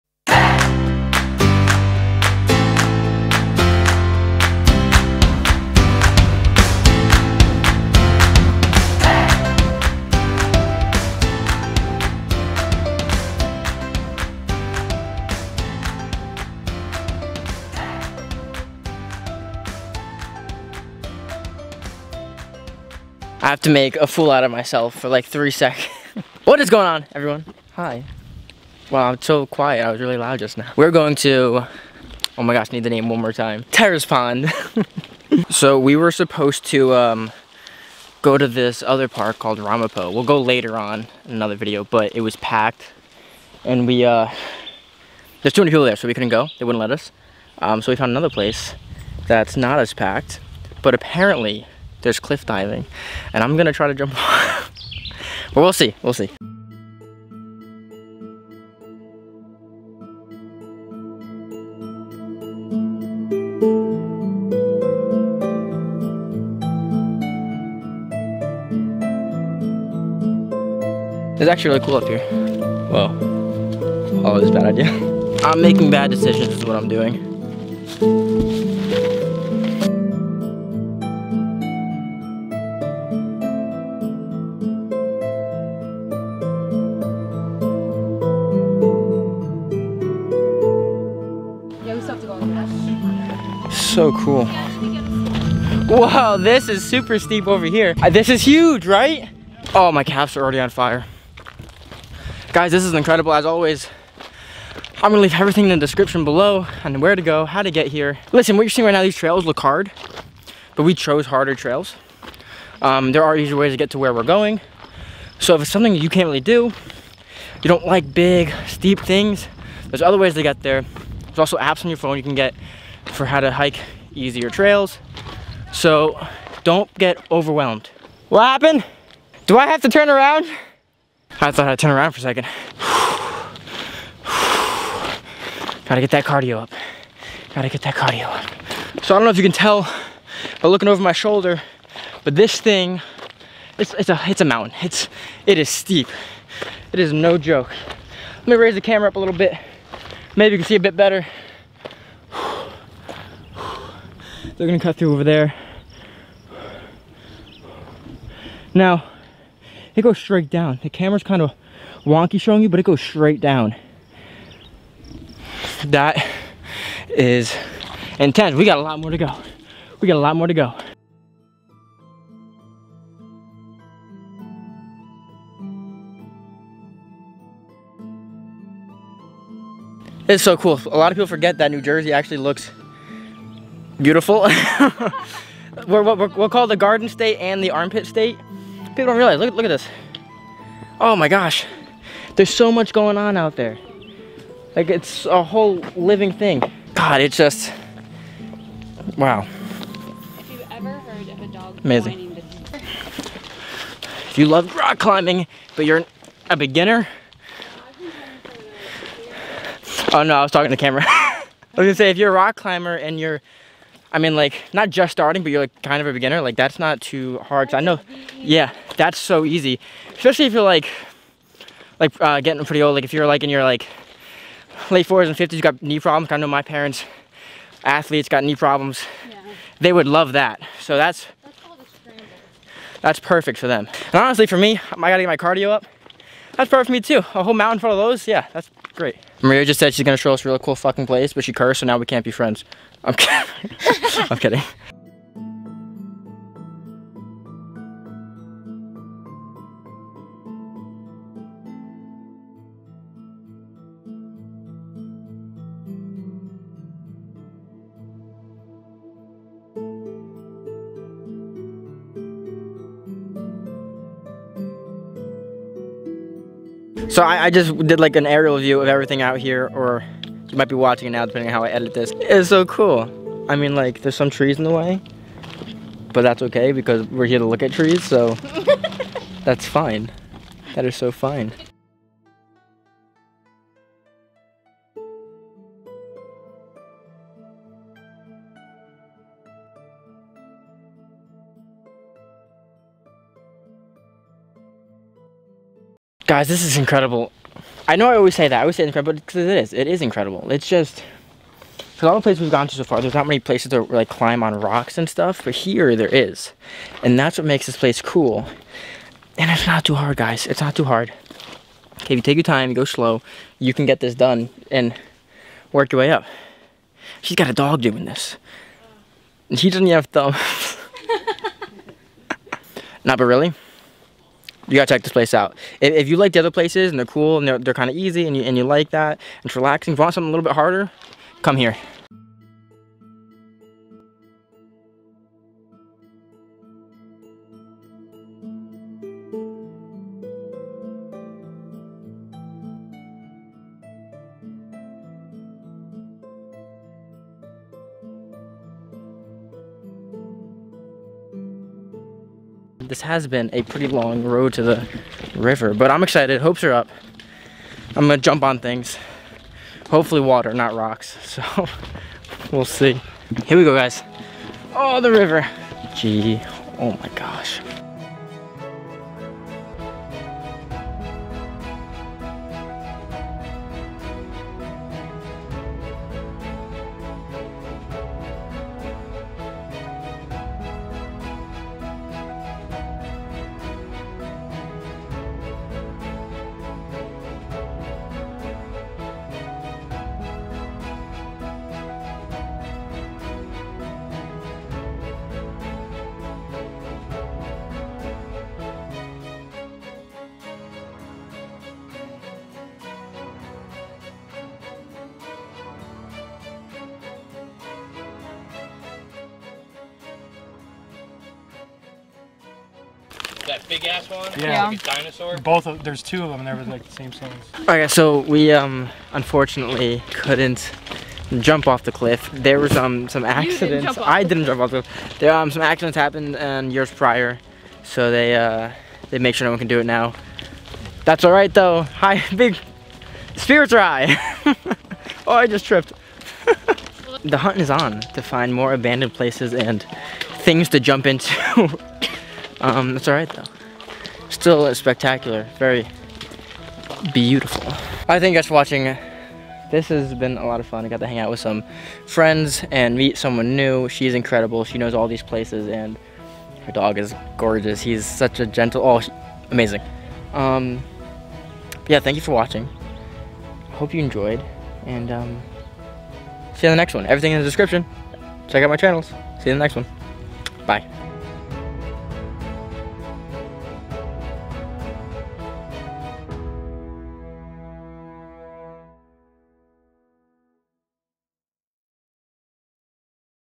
Our treks are getting steep: The hunt for cliff diving | Terrace Pond NJ
Trail